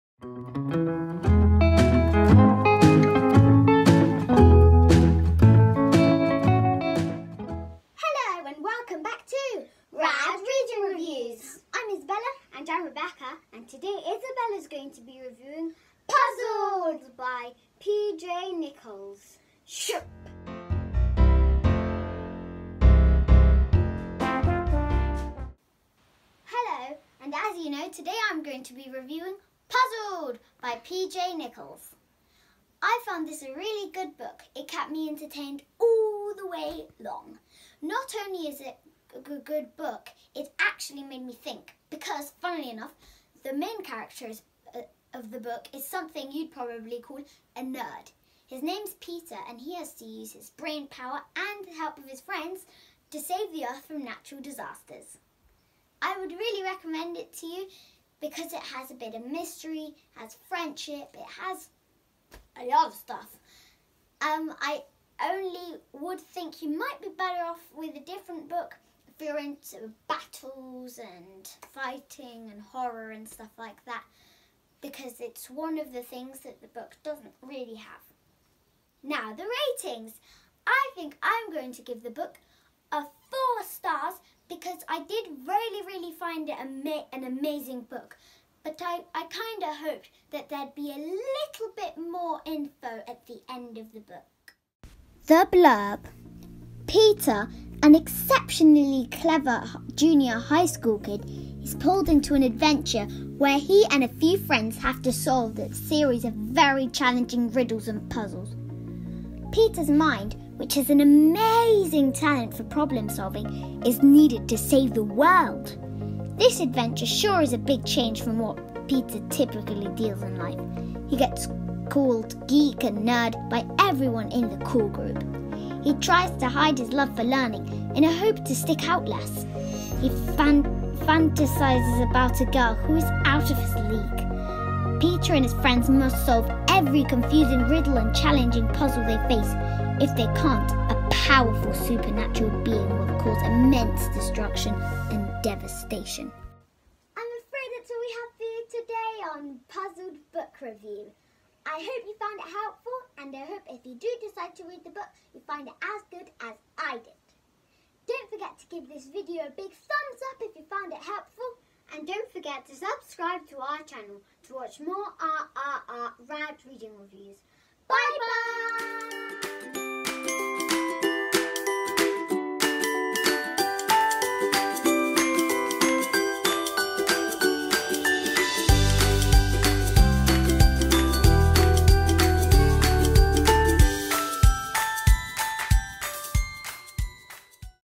Puzzled by P.J. Nichols book review
Isabella REALLY enjoyed this book. Here’s a review to help you decide whether you want to read it or not! 😃